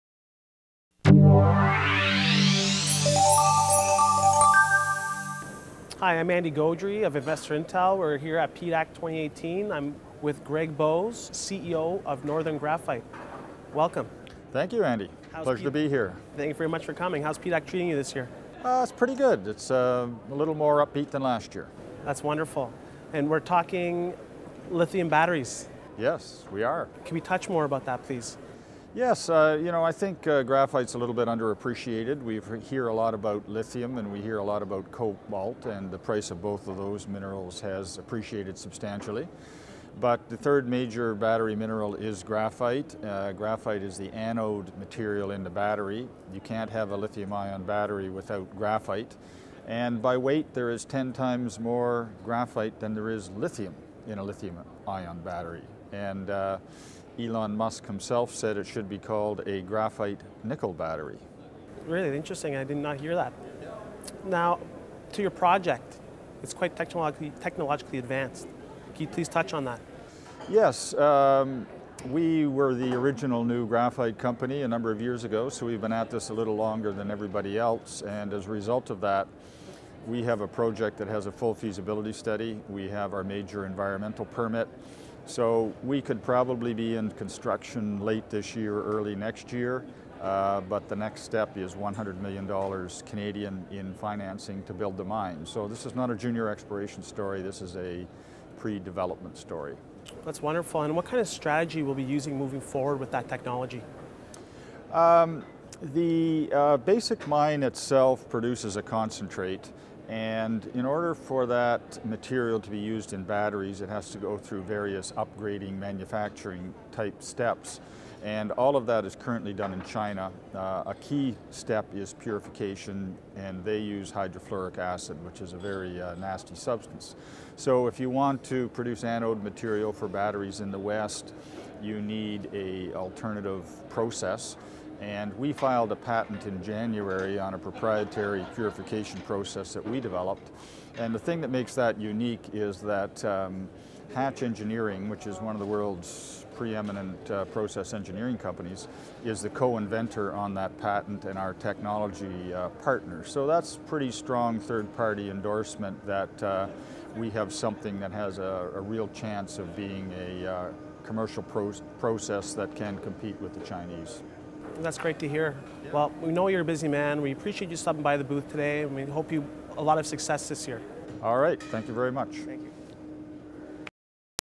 Northern Graphite CEO on their patent pending purification technology
March 23, 2018 – “We were the original new graphite company a number of years ago. We have been at this a little longer than everybody else. As a result of that we have a project that has a full feasibility study. We have our major environmental permit.” – states Gregory Bowes, CEO and Director of Northern Graphite Corp. (TSXV: NGC | OTCQX: NGPHF), in an interview with InvestorIntel’s Andy Gaudry.

Andy Gaudry: Thank you very much for coming. How is PDAC treating you this year?

Gregory Bowes: Pretty good. It is a little more upbeat than last year.

Andy Gaudry: That is wonderful. We are talking lithium batteries.

Gregory Bowes: Yes we are.

Andy Gaudry: Can we touch more on that please?

Gregory Bowes: Yes. I think graphite is a little bit underappreciated. We hear a lot about lithium and we hear a lot about cobalt. The price of both of those minerals has appreciated substantially. The third major battery mineral is graphite. Graphite is the anode material in the battery. You cannot have a lithium-ion battery without graphite. By weight there is 10 times more graphite than there is lithium in a lithium-ion battery. Elon Musk himself said it should be called a graphite-nickel battery.

Andy Gaudry: Really? Interesting, I did not hear that. Now to your project, it is quite technologically advanced. Can you please touch on that?

Gregory Bowes: Yes. We were the original new graphite company a number of years ago. We have been at this a little longer than everybody else. As a result of that we have a project that has a full feasibility study. We have our major environmental permit. We could probably be in construction late this year or early next year. The next step is $100 million dollars Canadian in financing to build the mine. This is not a junior exploration story. This is a predevelopment story.

Andy Gaudry: That is wonderful. What kind of strategy will you be using moving forward with that technology?

Gregory Bowes: The basic mine itself produces a concentrate. In order for that material to be used in batteries it has to go through various upgrading manufacturing type steps. All of that is currently done in China. A key step is purification and they use hydrofluoric acid, which is a very nasty substance. If you want to produce anode material for batteries in the West you need an alternative process. We filed a patent in January on a proprietary purification process that we developed. The thing that makes that unique is that Hatch Engineering, which is one of the world’s preeminent process engineering companies, is the co-inventor on that patent and our technology partner. That is pretty strong third-party endorsement that we have something that has a real chance of being a commercial process that can compete with the Chinese...to access the complete interview, click here